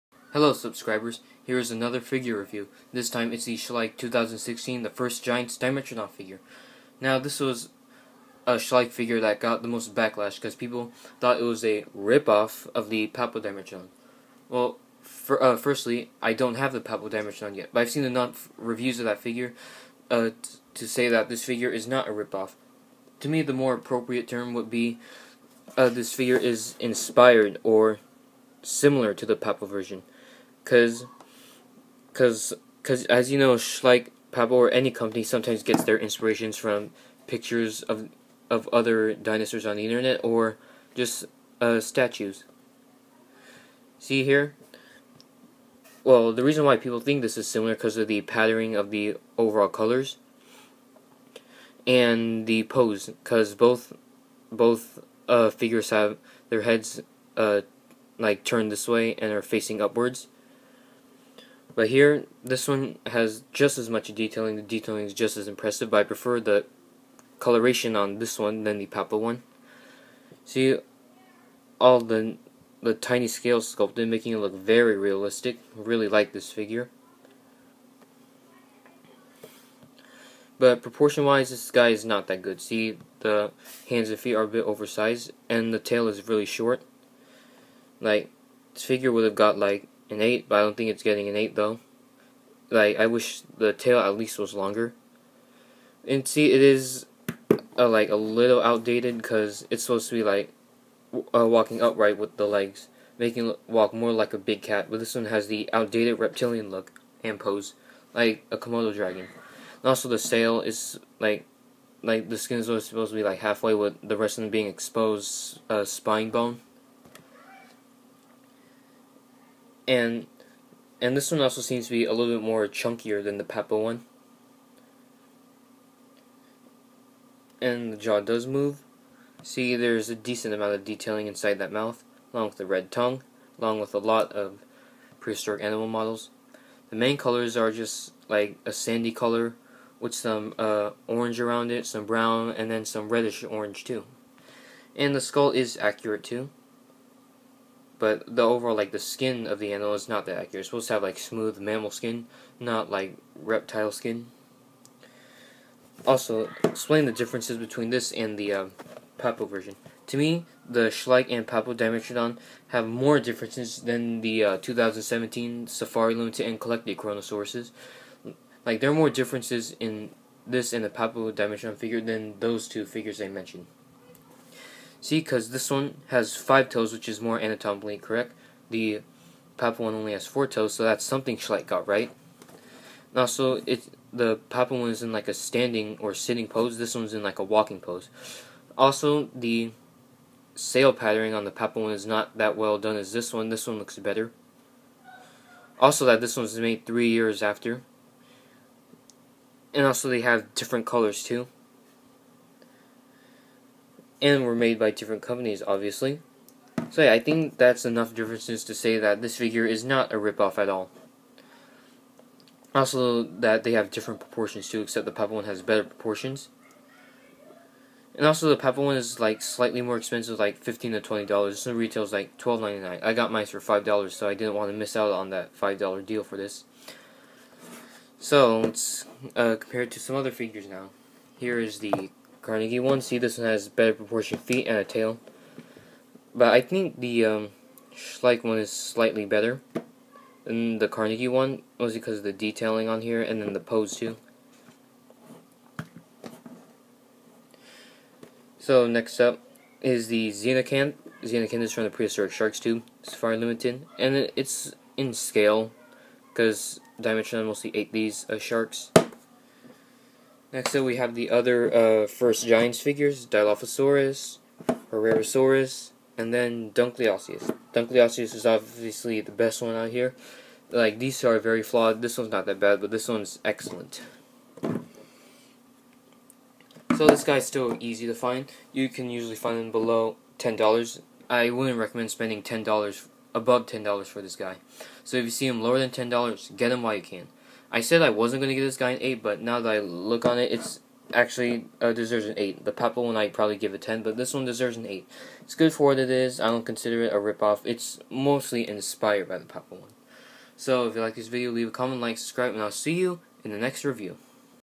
Schleich 2016 dinosaurs Dimetrodon figure review
An alright figure that gets an 8/10.

First off I'm probably one of those few people who think that this figure is NOT a ripoff of the Papo Dimetrodon. Not because I'm one of those biased Schleich fanboys, but because to me, I see plenty of differences between the two. People are probably saying that this is a ripoff because Schleich is a company that most people aren't too fond off, if Safari Ltd or CollectA made a Dimetrodon similar to Papo's, people probably wouldn't be complaining. Firstly, we don't even know if Schleich was ripping off Papo or was inspired by another image. I've seen enough reviews and photos of the Papo Dimetrodon to say that the Schleich one is not a ripoff. The Schleich Dimetrodon was made 3 years after the Papo one, not to mention the fact that they have completely different poses along with different paint jobs and sculpt detailing. The more appropriate term would be that Schleich's Dimetrodon is inspired by the Papo version. Also note that the two Kronosaurus from Safari and CollectA, they were released in the same year and have extremely similar poses, but no one is complaining, because they are only SIMILAR to each other with different color schemes.